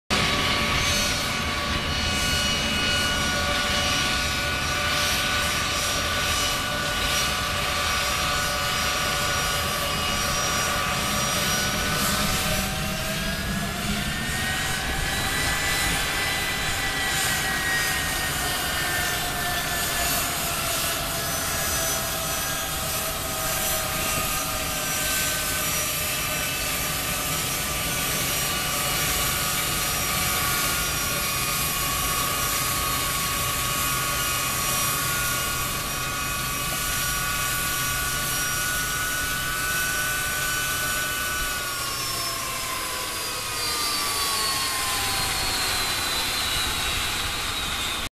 Boeing 757 taxi to gate
Boeing 757 taxiing into the gate @ SNA John Wayne Airport Santa Ana, ca